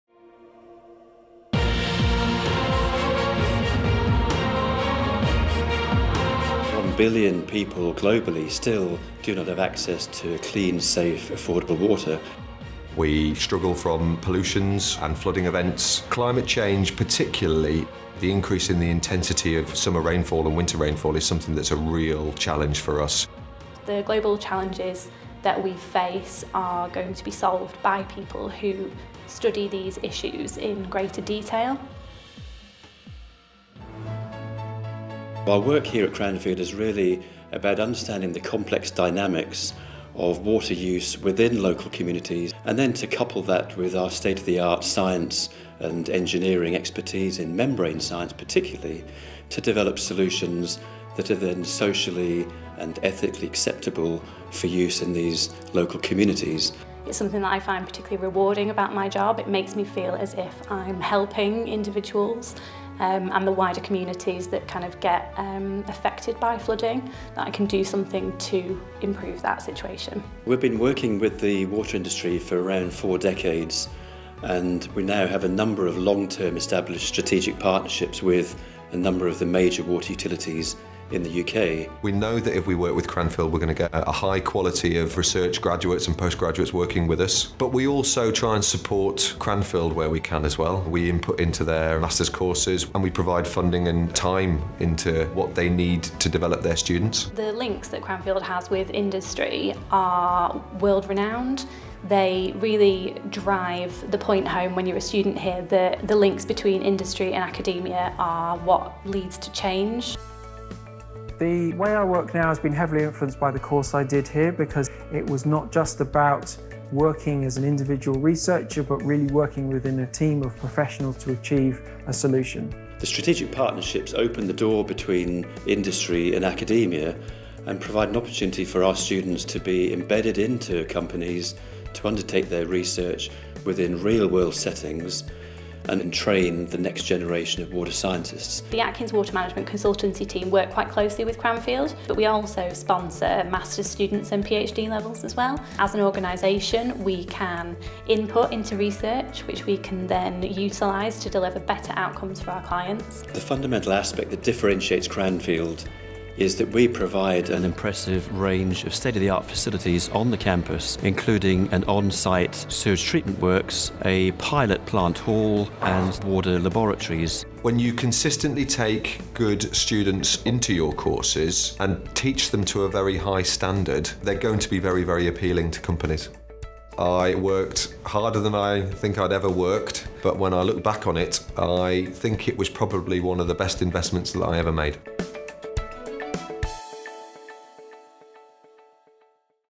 Water at Cranfield University
Our lives and livelihoods are dependent on the natural and engineered water cycles. Accordingly, research and skills development in water treatment and management has never been more vital. Cranfield’s research in these areas is applied and industry focused and we have been driving innovation for over 40 years.